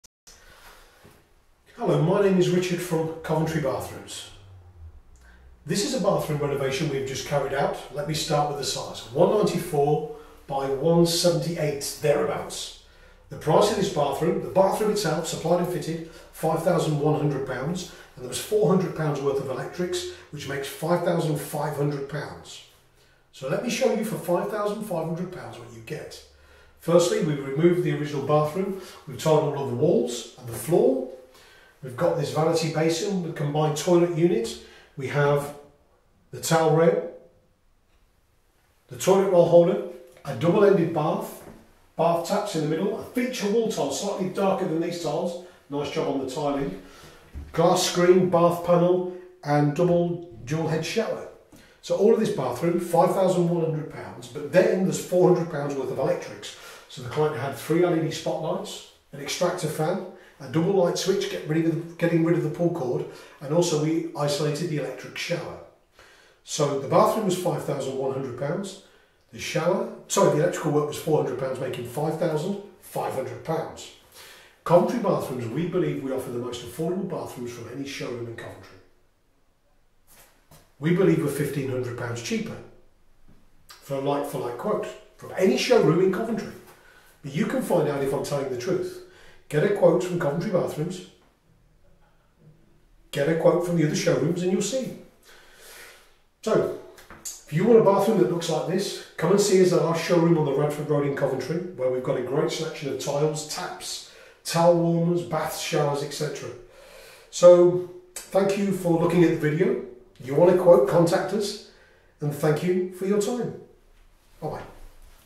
Bathroom renovation in Coventry Costing £5500
This is a bathroom renovation we carried out in Coundon in Coventry. The total cost of the bathroom supplied and fitted with all electrical work is £5500

The bathroom size is 194cm long by 178cm wide.  It would have been better if the bathroom was 194cm wide, never the less we have worked with the space we have available.
The clients requested a vanity basin, the only way we could fit a vanity basin into the space between the toilet and the bath was by fitting a combined basin and toilet unit measuring 95cm. This allowed for a space between the bath and the basin and more importantly between the toilet and the wall.
Included in the price
Removal of original bathroom and tiles with a skip to take all waste
Floor tiles, wall tiles and all tiling

Bath, bath panel, bath shower screen
Bath taps, basin taps
Dual shower running off the thermostat
95cm wide combined vanity basin and vanity toilet unit
Chrome towel warmer 500mm by 1600mm

Electrics included
3 LED ceiling down lights
Extractor fan
Change the orginal pull chord to a wall mounted double light switch.

All this for £5500

Bathrooms made affordable, designed correctly, professionally fitted.